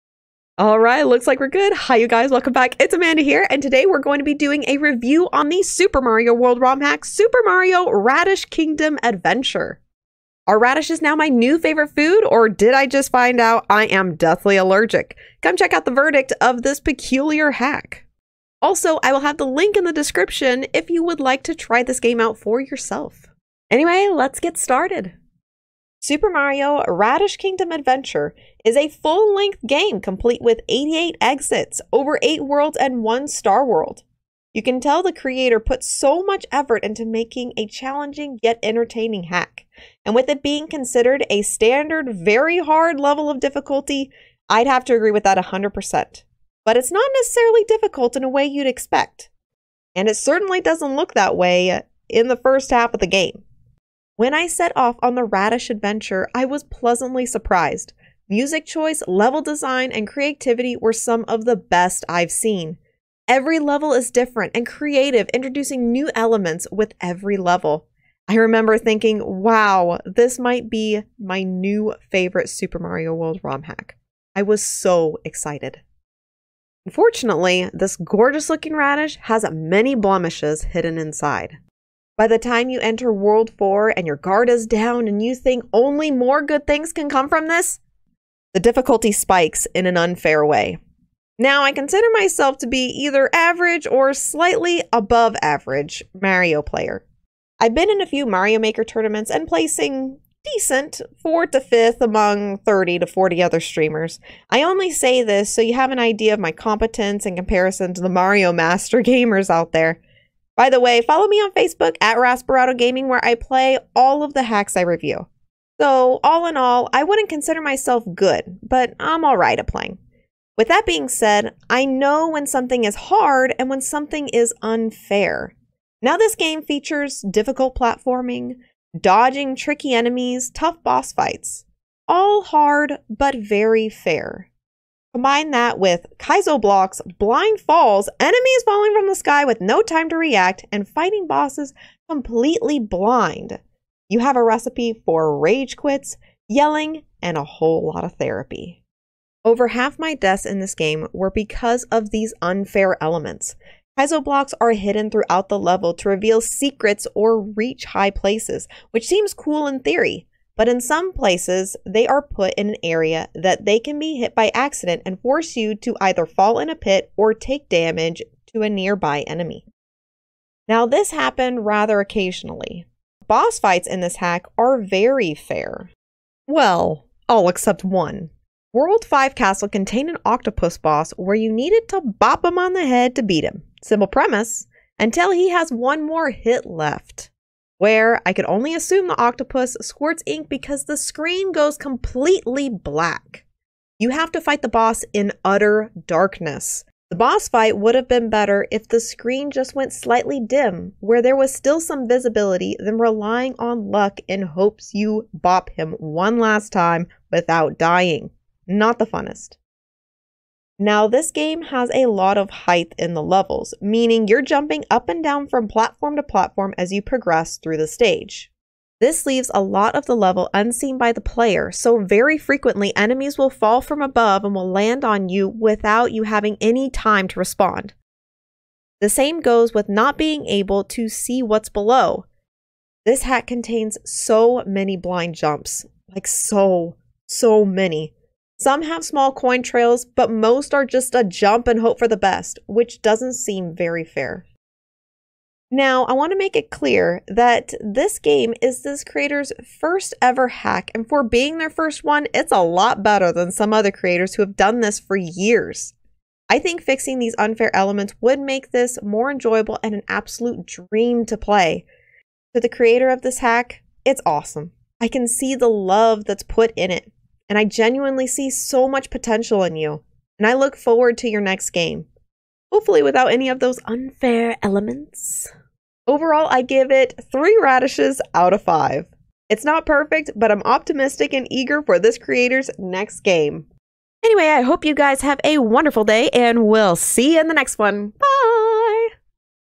Super Mario Radish Kingdom Adventure REVIEW!
Come check out the verdict of this Super Mario World ROM hack.